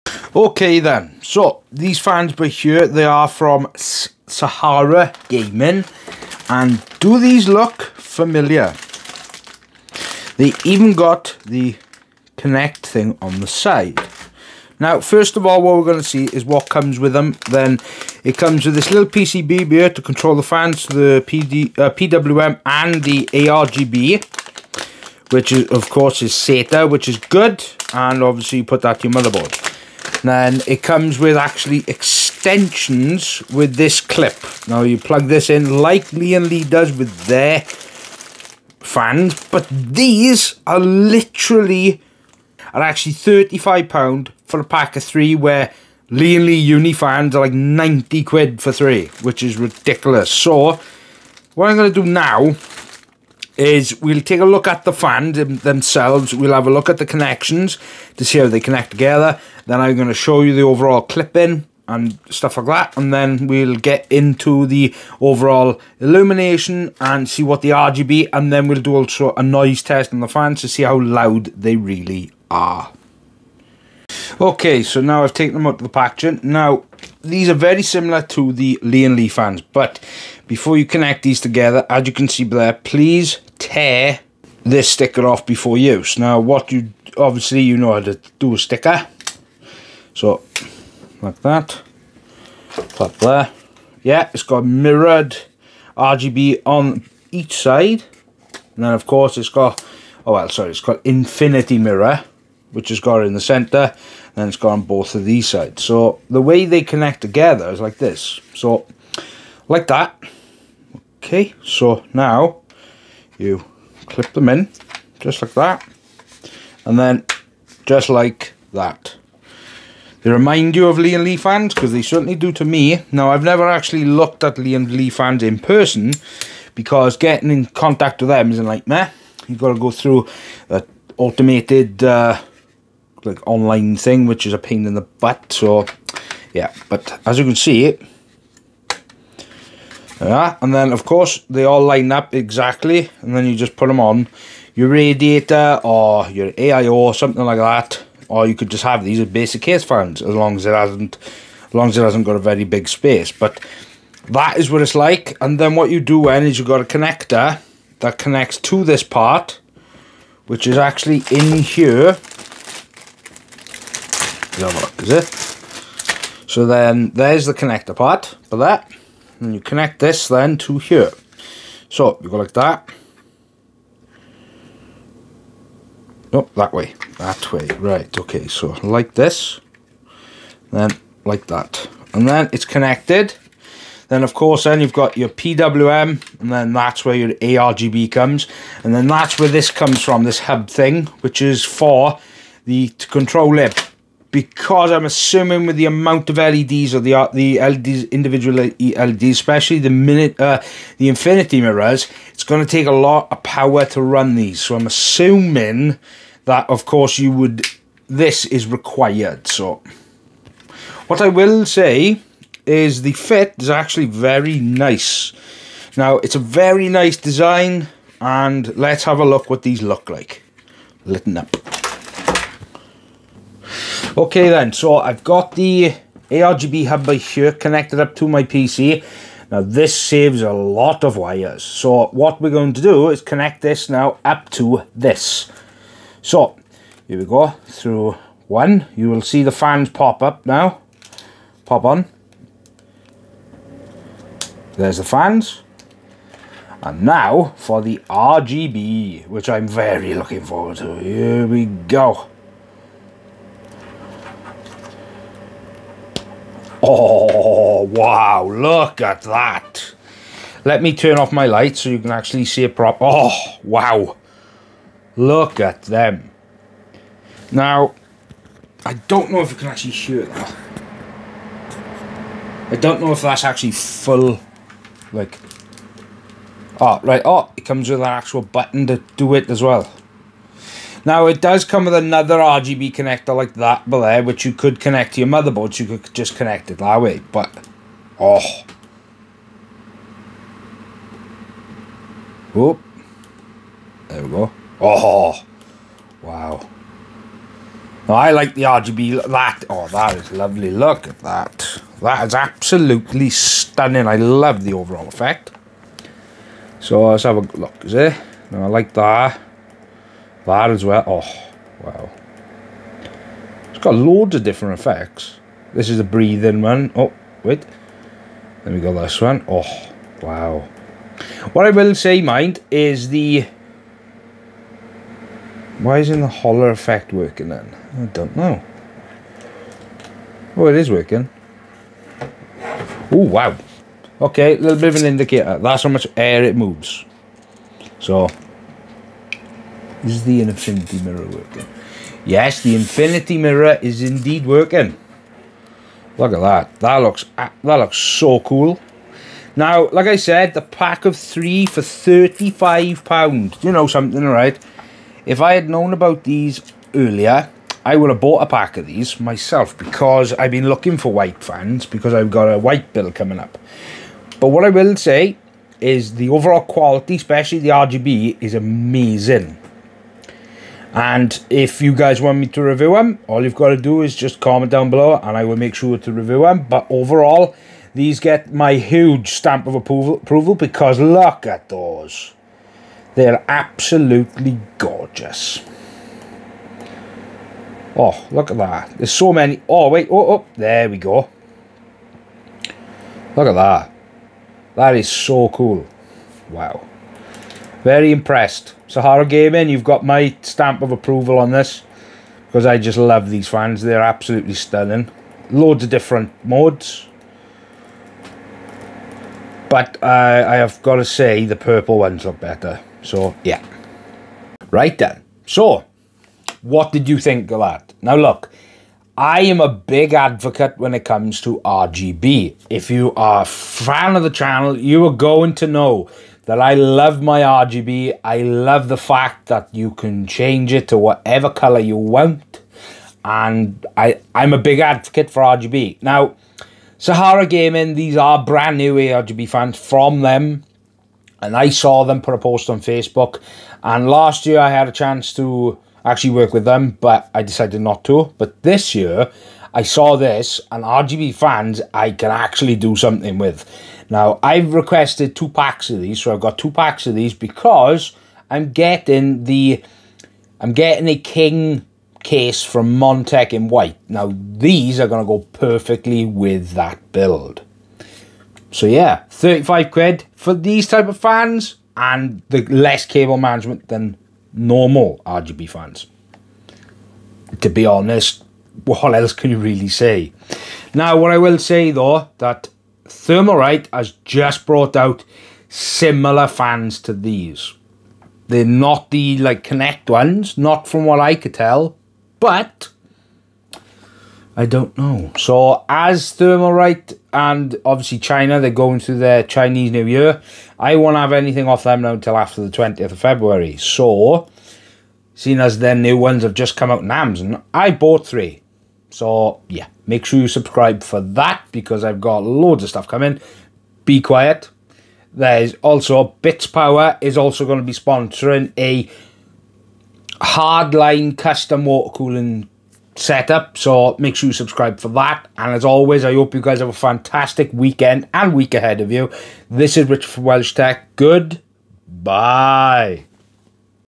The Best alternative to Lian Li UNIFANS ? / SaharaGaming M12WX3 White
Today's video SaharaGaming Brings you the best alternative Lian Li Unifans The MIRROR 12 WHITE X 3 & FAN HUB

We will Unbox these take a look at what comes with the fans and connect them together and show you what the ARGB Looks like

Excellent 120mm PWM fans from SaharaGaming. The Mirror120 fans have integrated addressable RGB LEDs and outstanding performance.
22 digitally addressable RGB LEDs per fan

The features of the SaharaGaming Mirror 12
The SaharaGaming Mirror 12 features a unique design, with the fans including contact pins and being connected via a plug-in PCB card system. Multiple fans no longer have to be connected individually, which saves a lot of time and prevents cable clutter.
Additionally a fluid dynamic ball bearings ensure smooth operation and improve durability. Since friction is reduced, the fans can run at higher speeds without greater energy consumption. The Mirror 12 fans have a maximum full speed of 1500 rpm with up to 27 dB (A) under full load. They also run quiet with 55 CFM airflow and 2.91 mm H2O static pressure at full speed
Each fan includes 32 digitally addressable RGB LEDs. This is operated via Motherboard SYNC software and enables users to set both fan speed and illumination effects. Alternatively you can use ANY SaharaGaming fan controller (hub) .

00:00 Introduction
00:19 What comes with the Fans
01:15 First look at the Fans and how they connect together
01:51 How they Connect
03:11 How are they Powered
03:43 What They look like With RGB on
04:27 Are they Loud ?
04:51 Trying different Colours
05:51 Cost for 3 Fans ?
06:03 How's the feel and quality
06:18 They have my stamp of approval
06:57 Should I review these & What do i think
08:31 Don't forget to Subscribe